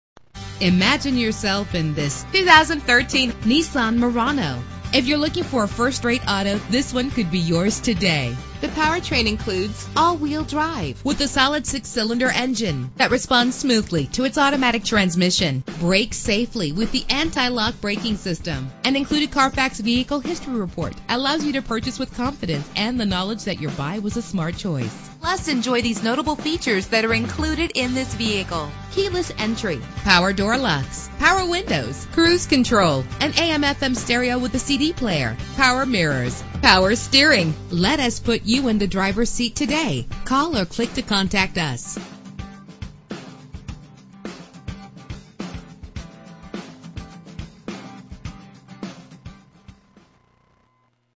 2013 Nissan Murano - Maplewood MN
Phone:
Year: 2013
Make: Nissan
Model: Murano
Trim: SV
Engine: 3.5 liter 6 cylinder 24 valve MPFI DOHC
Transmission: Automatic CVT
Color: Brilliant Silver
Mileage: 40166
Address:
3090 N. Hwy 61